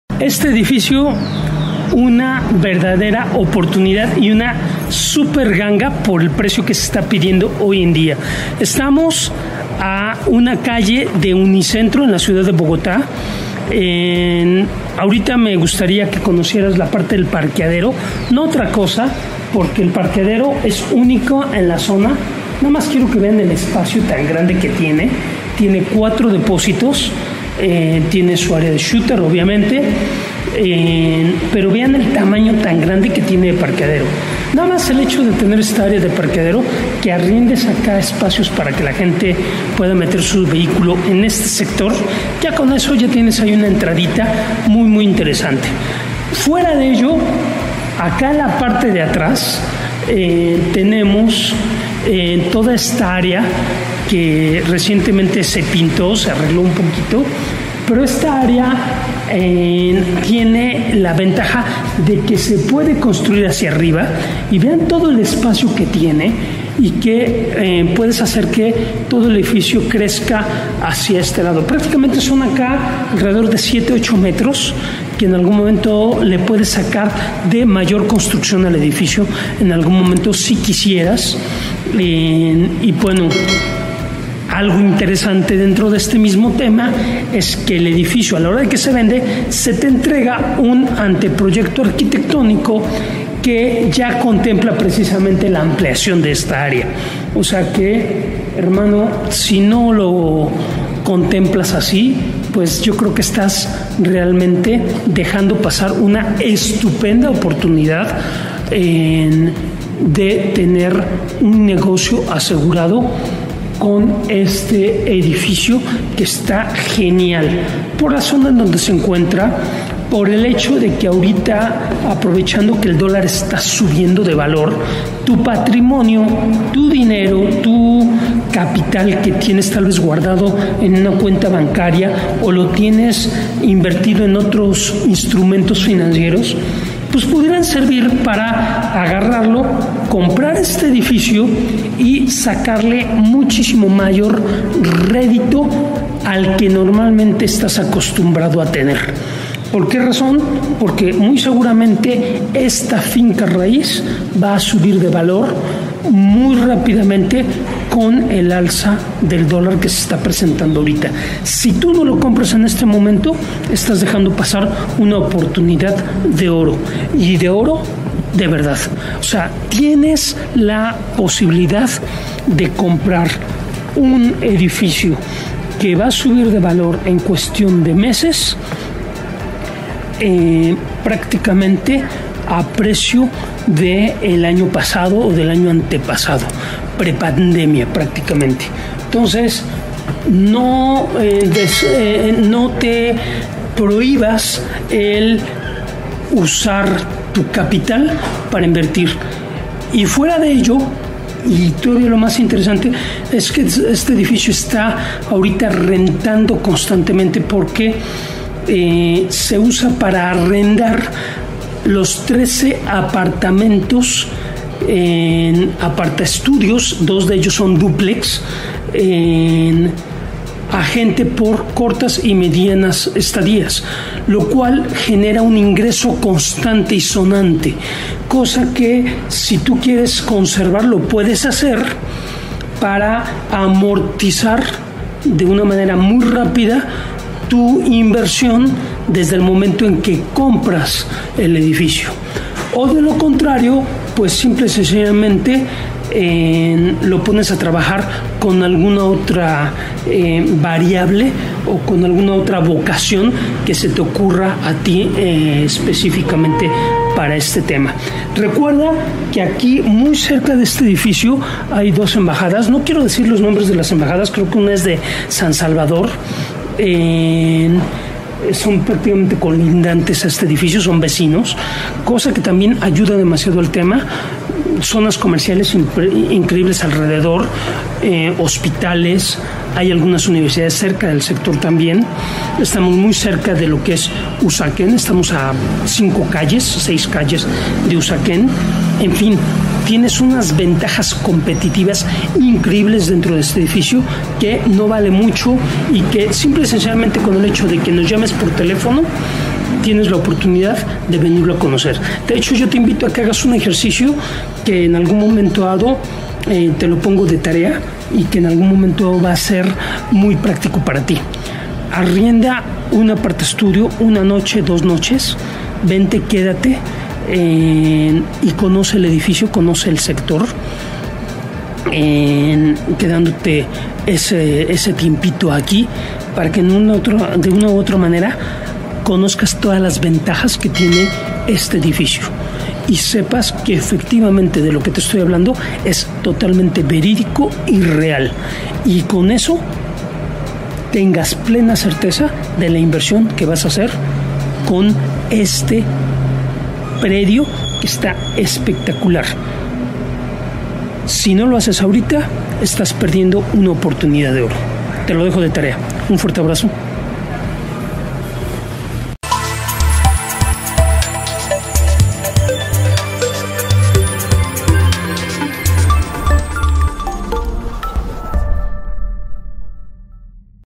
Es la mejor hora de invertir tu patrimonio económico en COLOMBIA con esta excelente oportunidad
• Adquiere este edificio ubicado estratégicamente en la ciudad capital de Colombia, y empieza a hacer crecer tu dinero con esta valiosísima oportunidad que te brindara la posibilidad de multiplicar tu patrimonio, es el mejor momento para hacerlo, al aprovechar que tus dólares valen mucho más!!!

EDIFICIO APARTAMENTOS AMOBLADOS
CASA REGIS USAQUÉN BOGOTÁ D.C.
DESTACADO:
13 Apartamentos: 3 Junior: 1 Habitación, Baño, Cocina Comedor – 35M2 • 8 Ejecutiva: 1 Habitación, Baño, Cocina Comedor - 45M2 • 2 Master: 3 Habitaciones, Baños, Cocina, Sala Comedor, Dúplex – 156M2 Recepción 24 horas Áreas comunes Wi-Fi 13 Parqueaderos cubiertos Área lavadero común Área del lote: 405M2 Área Construida: 1,194M2
El edificio se construyó en el año 1.976 originalmente varios apartamentos de una misma familia, parte de los cuales son los propietarios actuales. Posteriormente, se integró en un solo inmueble y se desarrolló el ApartaSuites. La familia propietaria se ha dedicado al sector de hotelería de cortas y medianas estadías desde antes de la creación del hospedaje. Este Aparta-hotel es categoría 3 estrellas y se encuentra en una zona muy exclusiva, cerca de varias embajadas, a una cuadra de Unicentro, el principal centro comercial de Bogotá. En una calle de muy poca circulación. Las áreas comunes están adecuadas para sentirse como en casa. No es Propiedad Horizontal. Se vende a puerta cerrada: 100% equipado.
UBICACIÓN:
Localidad: Usaquén Barrio: Santa Bárbara Central Estrato: 6 Zona Residencial y Comercial Centro Comercial Unicentro: 5 minutos a pie Centro Comercial Santa Bárbara: 15 minutos a pie Centro Comercial Santa Ana: 20 minutos a pie Embajadas de Costa Rica, El Salvador, Honduras, a pocos pasos Embajada de Canadá: 15 minutos a pie Embajada de Alemania: 15 minutos a pie Club de Campo Country Club: 15 minutos a pie Mercado de Pulgas Usaquén: 25 minutos a pie Fácil acceso a principales vías como las carreras 9, 15 y 19; Autopista Norte, Avenida 7, Calle 127, salida al Aeropuerto El Dorado Salida rápida al Norte de Colombia A pocas cuadras de la Clínica Reina Sofía, Clínica La Carolina y principales Centros Médicos y Empresas de Salud Centro de Convenciones AR: 20 minutos a pie Universidad El Bosque: 25 minutos a pie Hospital Santa Fe de Bogotá: 15 minutos a pie Aeropuerto El Dorado: 18 km
RENTABILIDAD:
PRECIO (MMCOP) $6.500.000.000
INGRESOS * (MMCOP) $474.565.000 ION (**) $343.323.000 RENTABILIDAD (%) 6,24 | (*) Ingresos Anuales, considerando 70% Ocupación (**): Ingreso Operacional Neto | SIMULACIÓN CRÉDITO HIPOTECARIO • PRECIO (MMCOP) $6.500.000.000 CRÉDITO (70%) $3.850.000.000 CUOTAS (20 AÑOS) 240 TASA INTERÉS E.A. 8,7 % CRÉDITO (70%) $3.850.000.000 VALOR 1ra. CUOTA $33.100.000 Incluye: Seguros de vida, de incendio, terremoto, estudio del crédito y avalúo (Valores suministrados por el dueño del edificio)

Atención inversionistas de estadosunidos o europa interesados en colombia •